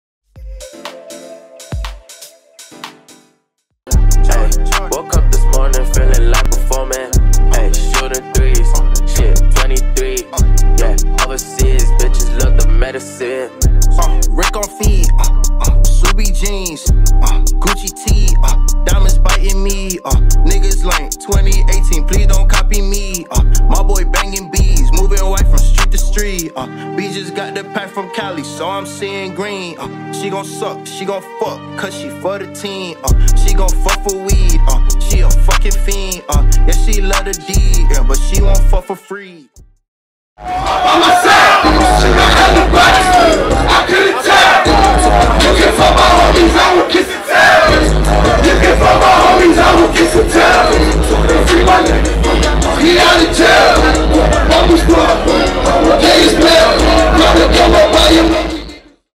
Traplanta @ Everything Pretty
Traplanta performing at Prettyboy D-O's  'Everything Pretty' festival in Lagos, Nigeria.